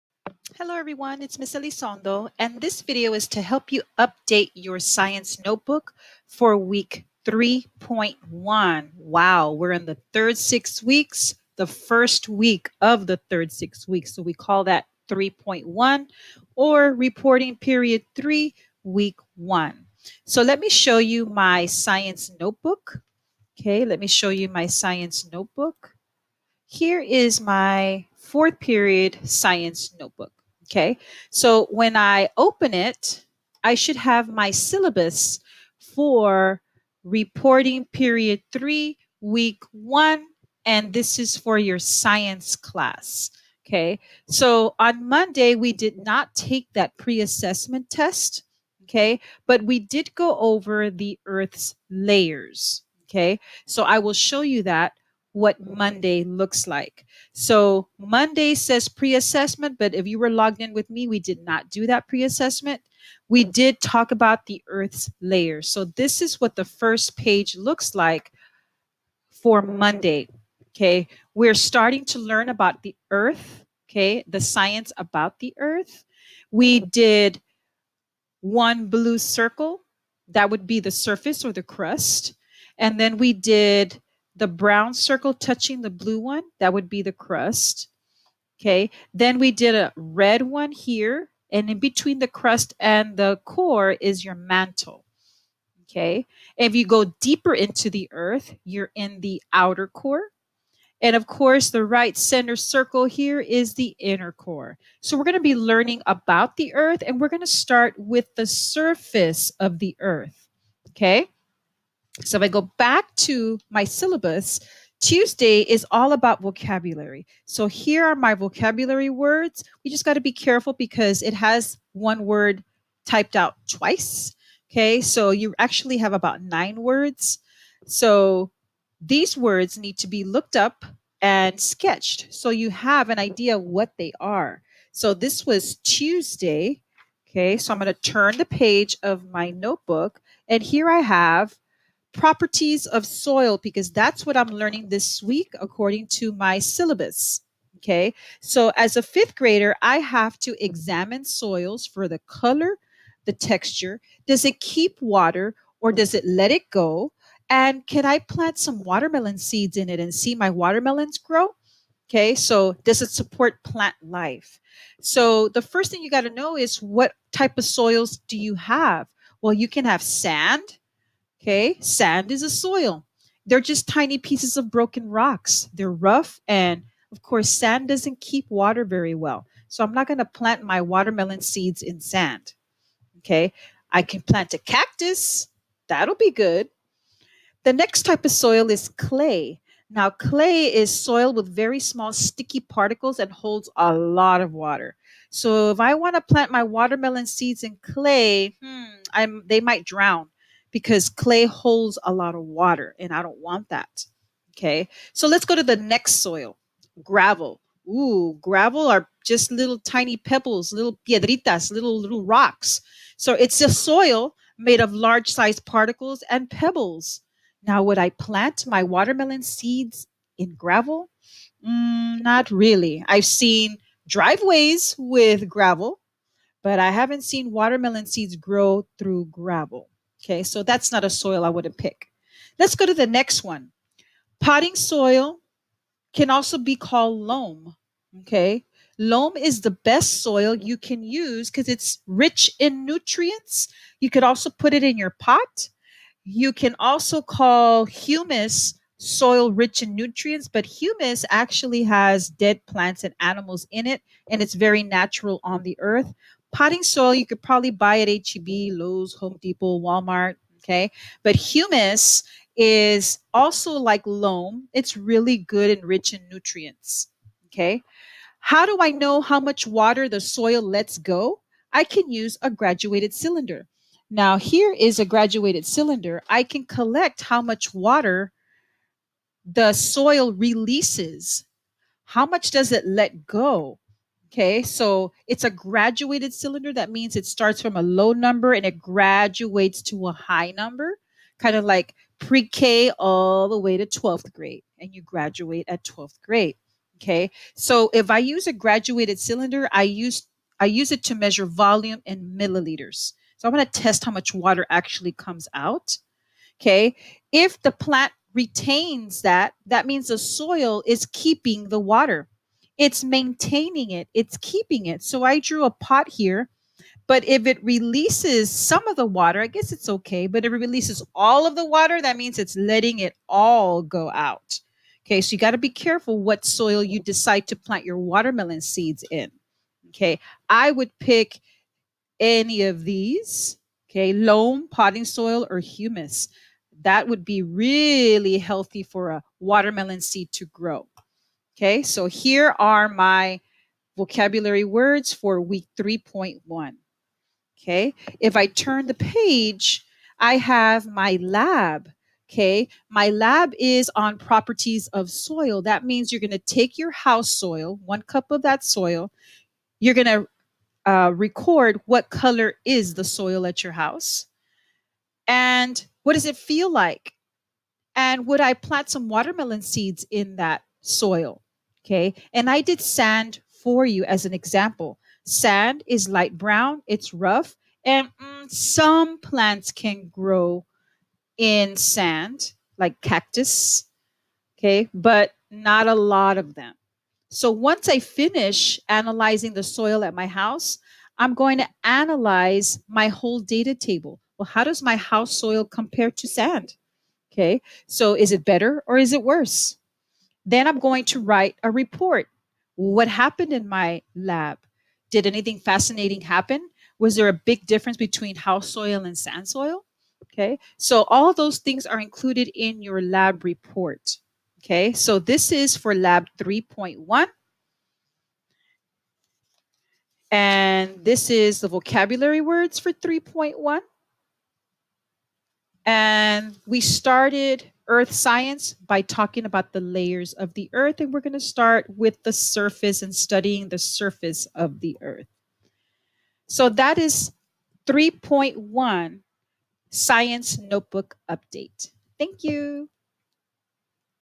3.1 Science Notebook Update
This video will help you update your science notebook for week 3.1 Properties of Soil.